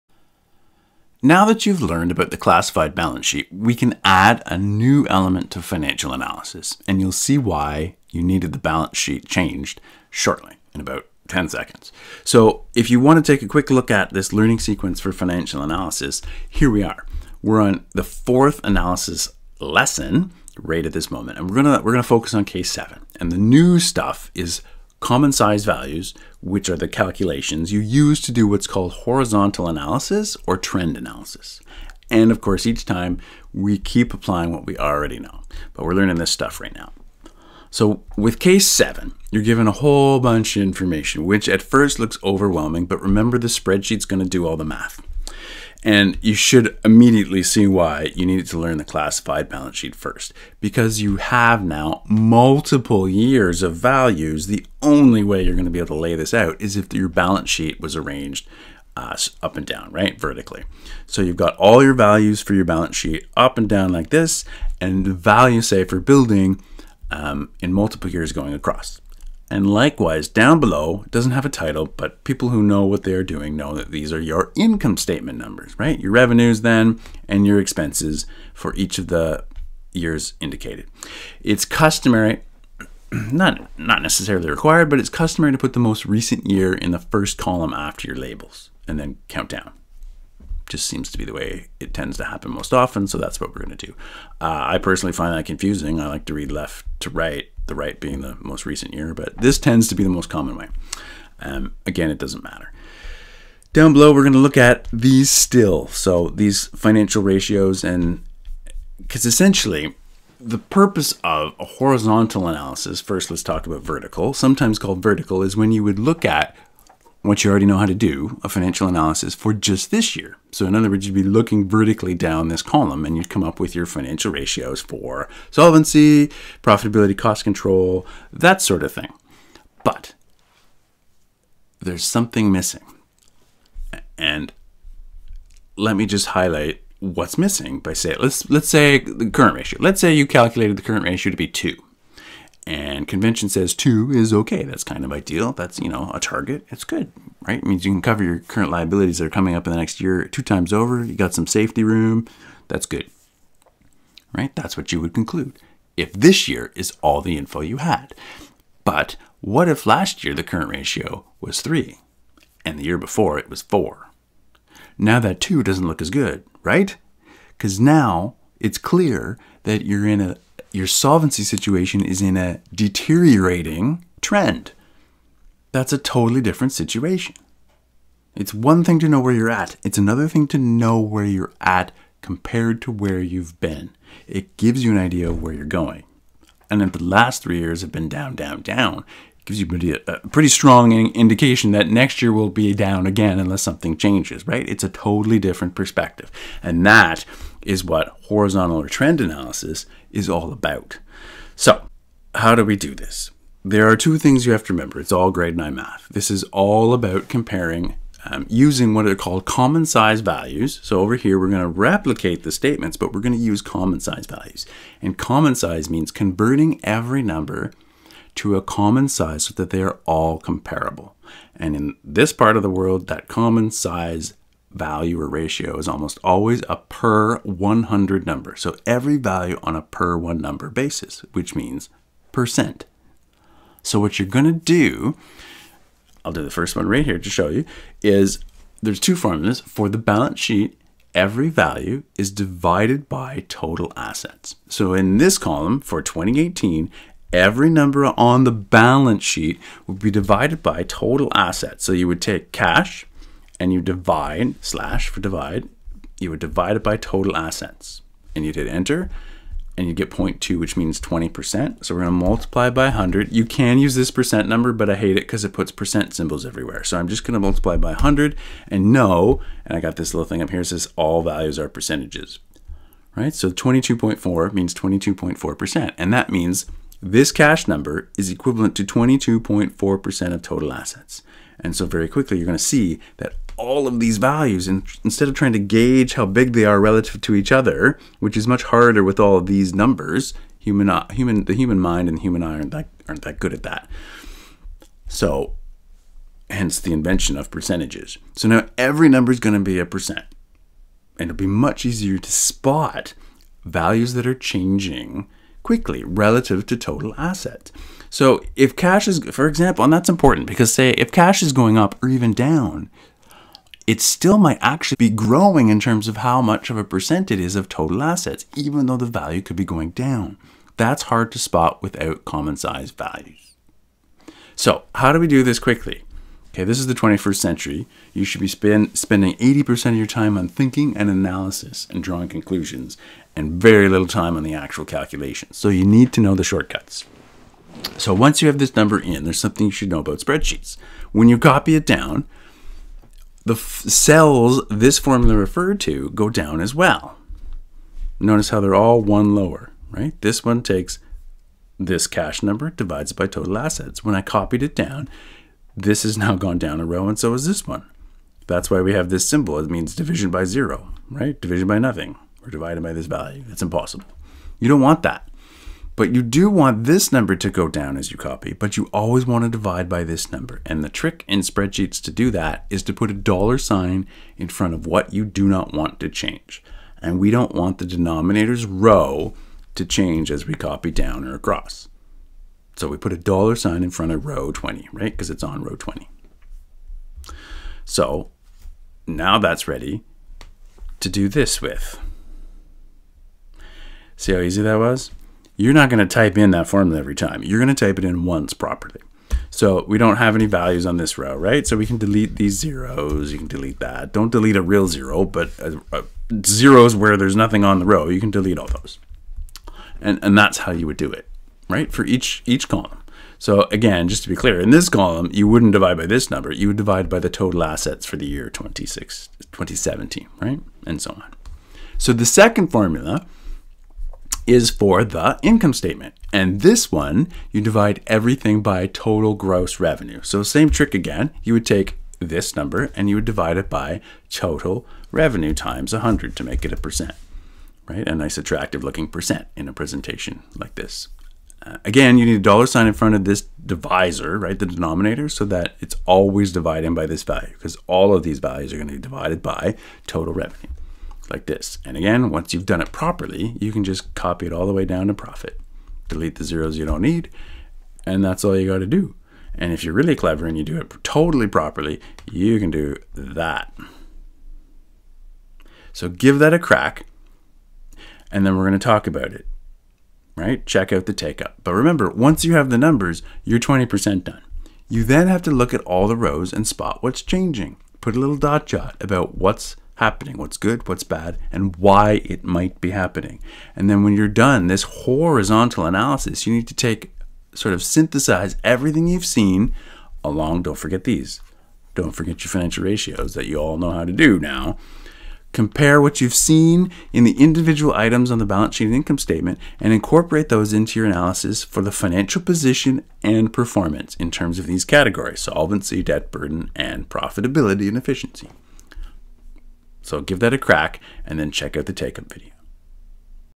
Analysis 4: Common Size Values
This is a quick overview of how to calculate common size values so that you can complete a horizontal/trend analysis of financial statements.